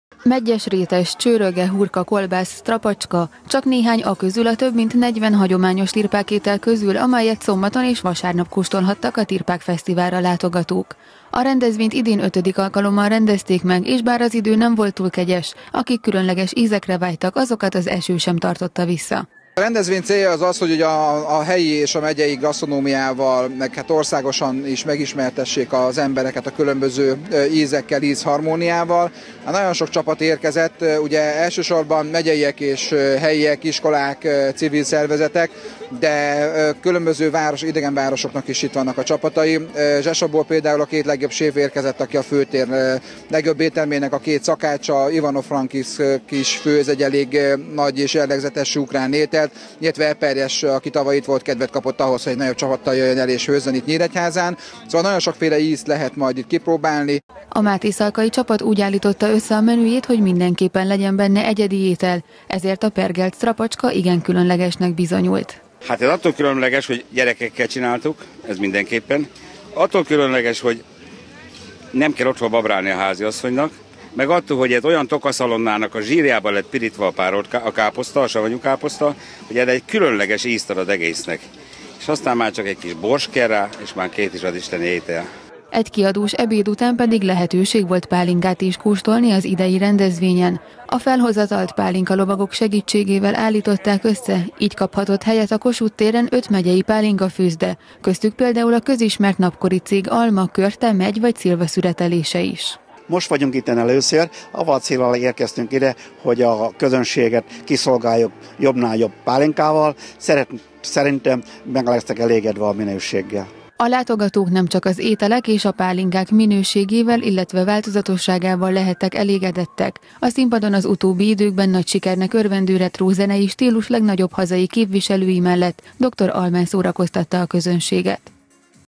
Tirpák Fesztivál - Kölcsey Tv
Több mint 20  csapat résztévtelével, idén már 5. alkalommal szerveztek Tirpák Fesztivált Nyíregyházán. A hétvégén két napon keresztül közel 40 féle étel és több pálinka közül válogathattak a Kossuth térre látogatók. A szombat este egy retro koncerttel zártult.